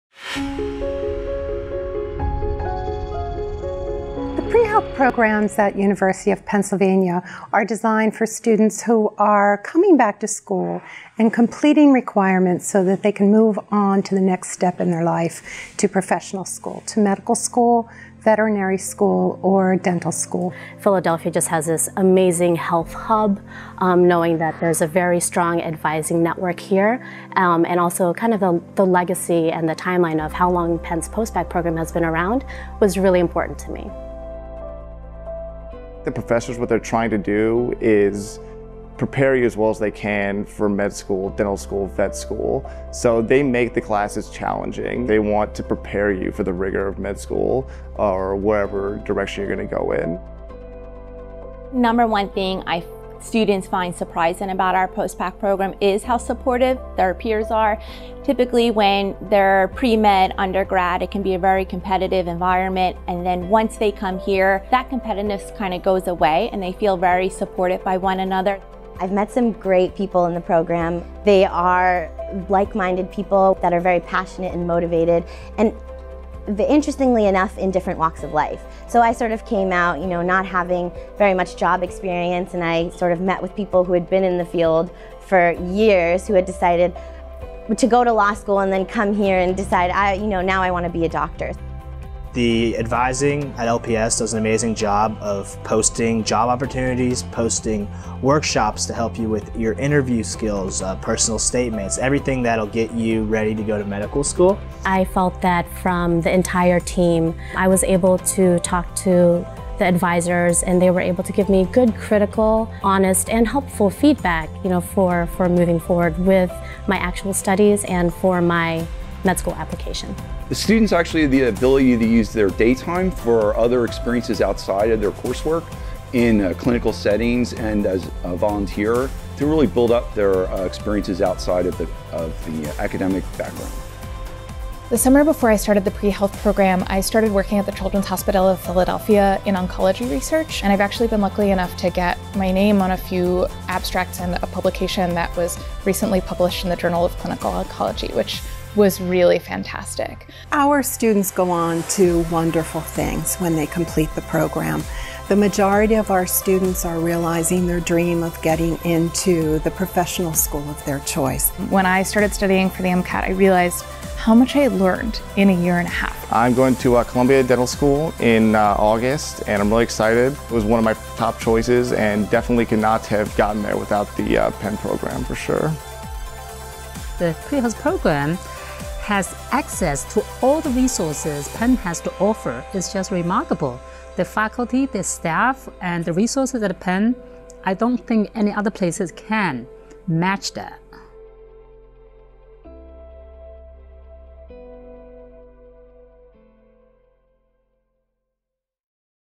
Penn's Pre-Health Programs: Open the doors to success
Penn’s Pre-Health Programs provide students a wide array of opportunities to jumpstart their training and careers in the professional health fields. Hear from the program director, students, alumni and faculty about all that Penn and its leading medical research centers have to offer.